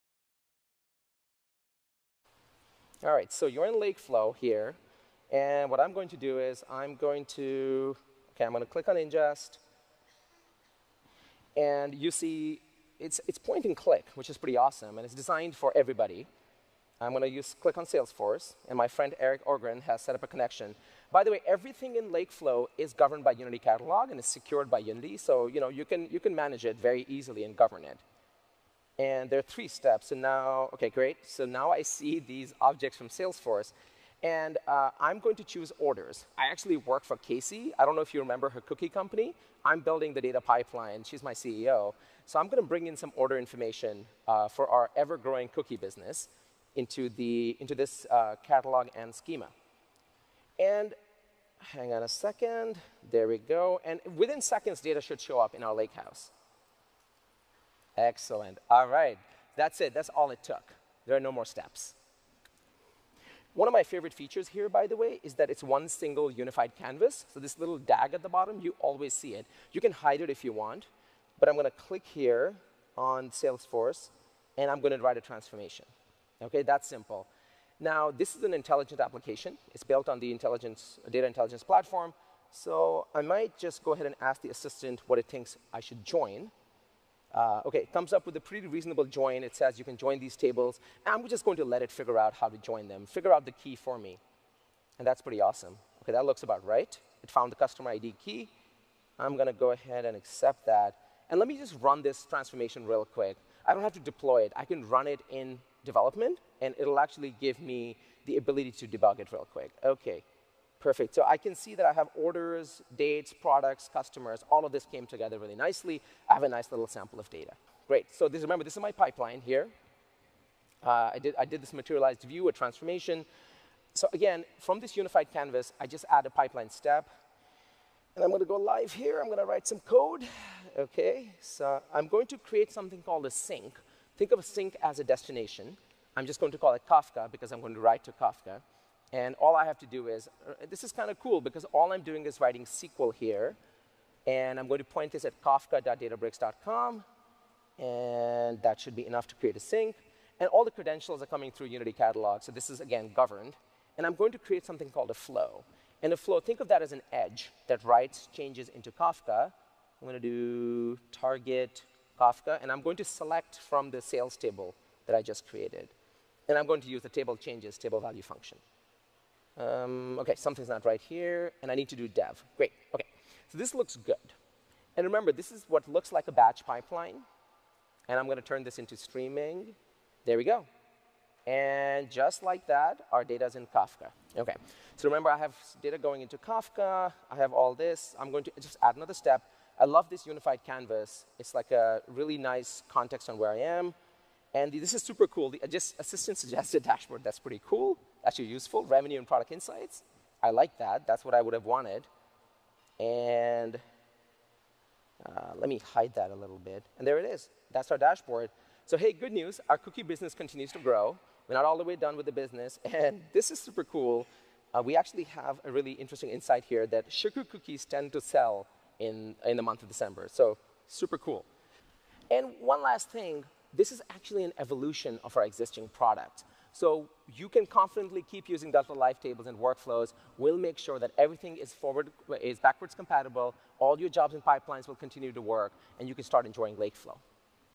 LakeFlow Demo
Databricks LakeFlow is a new solution that contains everything you need to build and operate production data pipelines. It includes new native, highly scalable connectors for databases including MySQL, Postgres, SQL Server and Oracle and enterprise applications like Salesforce, Microsoft Dynamics, NetSuite, Workday, ServiceNow and Google Analytics. Users can transform data in batch and streaming using standard SQL and Python.